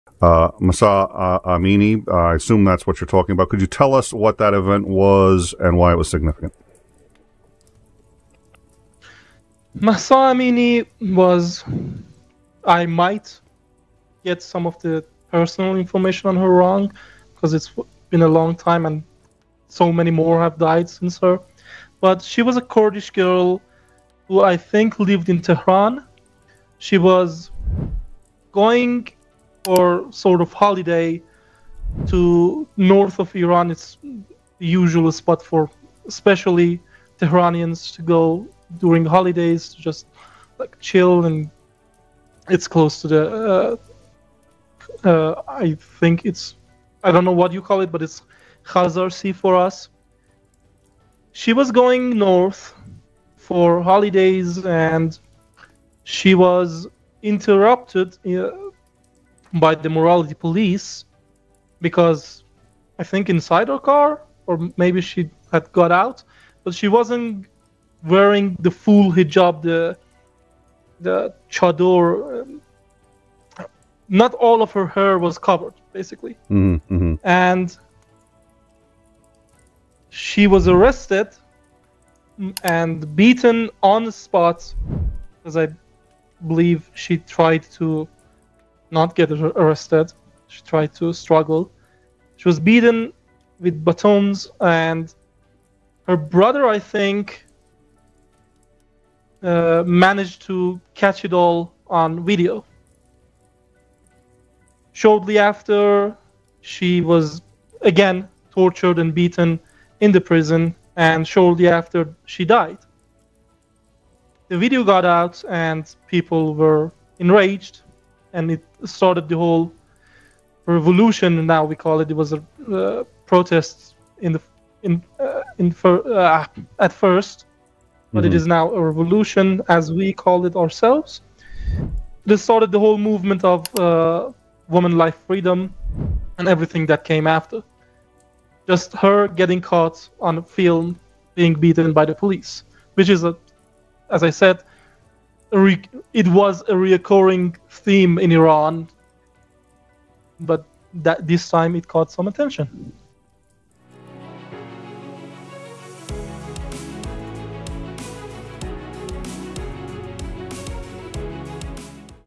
Mahsa Amini: Uncovering the cause of death | Iranian Protests | Alex Sun
In Time offers a unique perspective on news, politics, and culture. Join us for political discussions, debates, and expert opinions.

𝐍𝐢𝐜𝐡𝐨𝐥𝐚𝐬 𝐋𝐚𝐦𝐚𝐫 𝐒𝐨𝐮𝐭𝐭𝐞𝐫
📕 The Water Thief (Book)